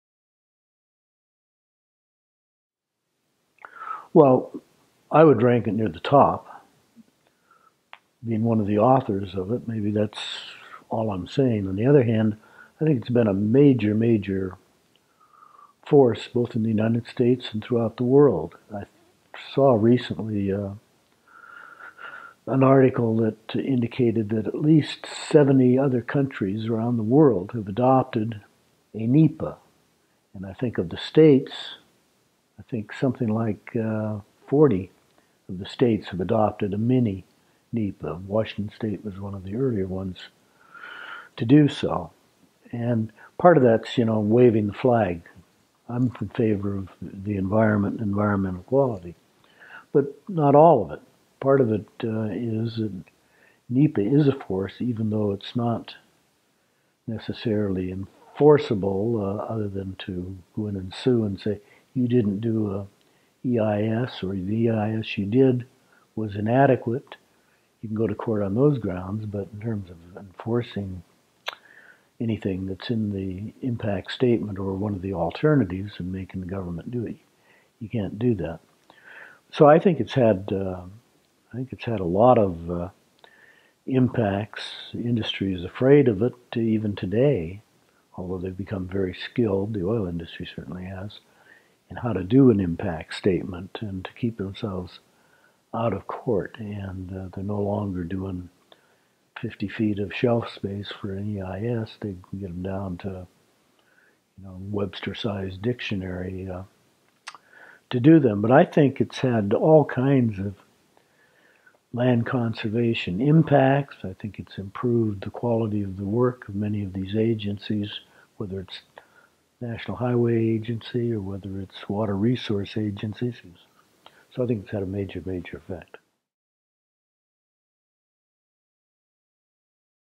Bill Van Ness: NEPA's Legacy
Bill Van Ness, Chief Counsel to the U.S. Senate Committee on Interior and Insular Affairs (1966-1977) talks about the legacy of the National Environmental Policy Act sponsored by Senator Henry M. Jackson.